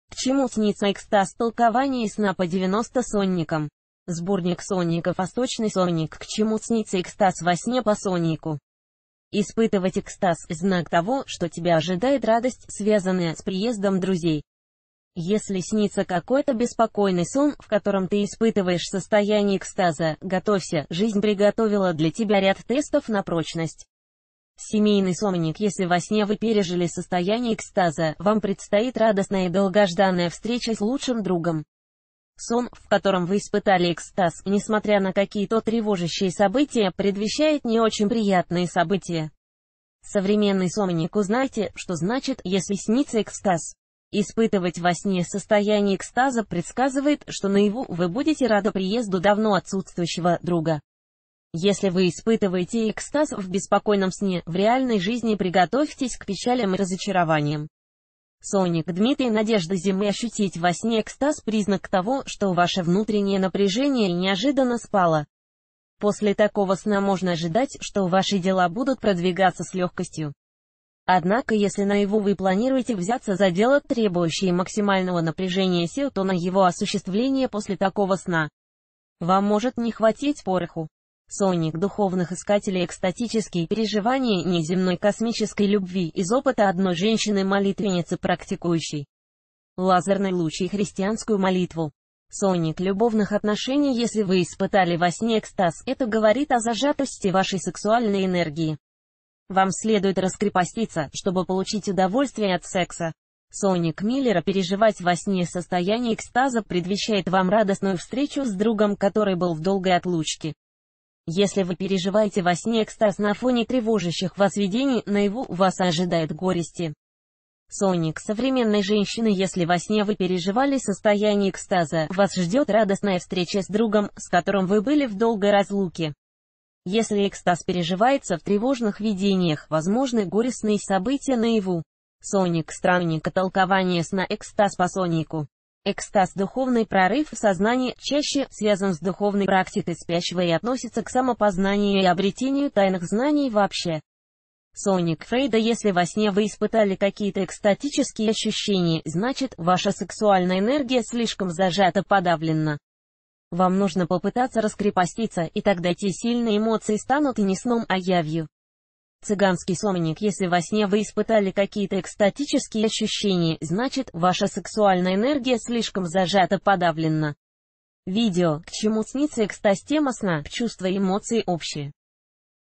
«Экстаз к чему снится во сне? Если видишь во сне Экстаз, что значит?»
Хотите узнать к чему  снится Экстаз во сне? Смотрите что значит, если видишь во сне  Экстаз по соннику абсолютно  бесплатно!